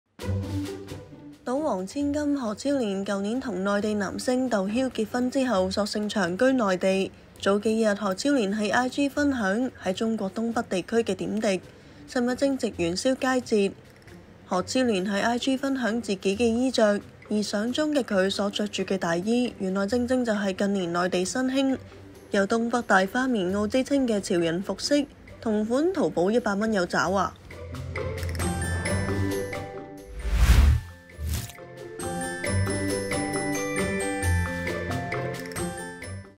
「賭王千金」何超蓮潮着東北大花棉襖 同款淘寶有售100蚊有找｜01娛樂｜何超蓮｜東北｜淘寶｜潮人
「賭王千金」何超蓮近年將生活及工作重心轉往內地，去年與內地男星竇驍結婚後更索性長居內地。早幾日何超蓮在IG分享在中國東北地區的點滴，寫着：「南方小土豆從29度的地方跑到零下20幾度的北方」。